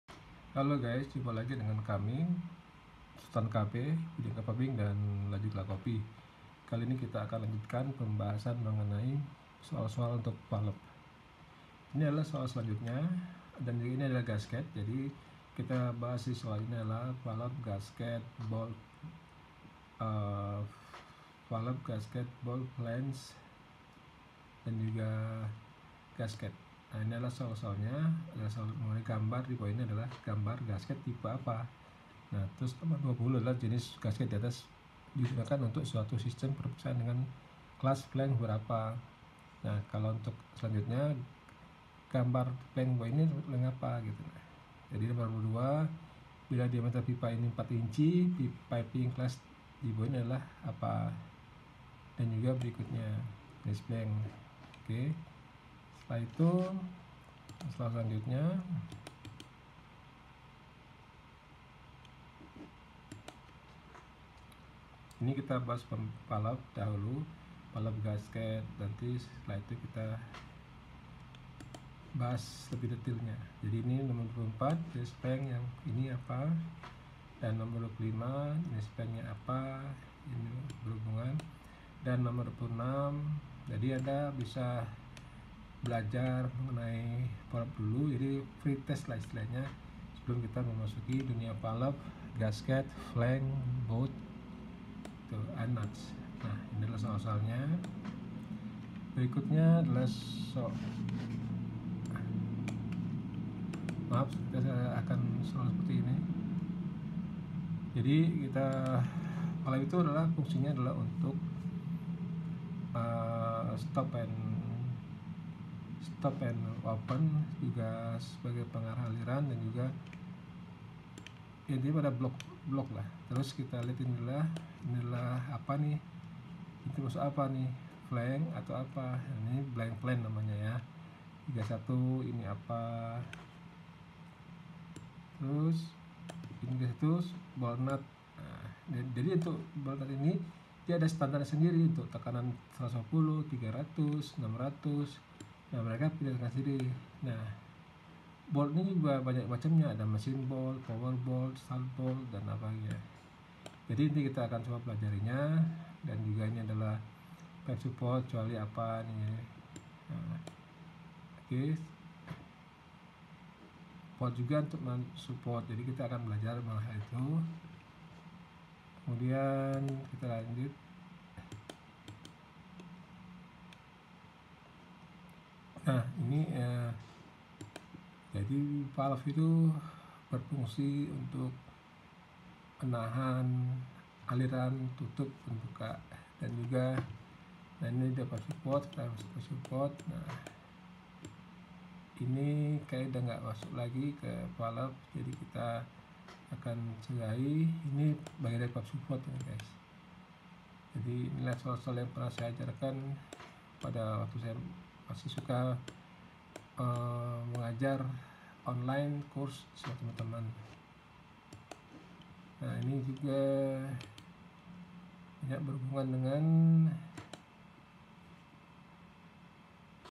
BELAJAR VALVE LEWAT SOALNYA-SOAL PART 2
Valve fungsinya adalah untuk stop and open aliran, juga sebagai pengarah aliran dan banyak jenis-jenis valvenya, juga basket, batu, flange,. Jadi sementara belajar lewat soalnya dulu. Ada bagian mengenai supporto pada soal ini Tapi nanti kita akan belajar juga.
Ini soal dari No 19-39 jadi di jawab aja dan besok kita Bahasa soalnya.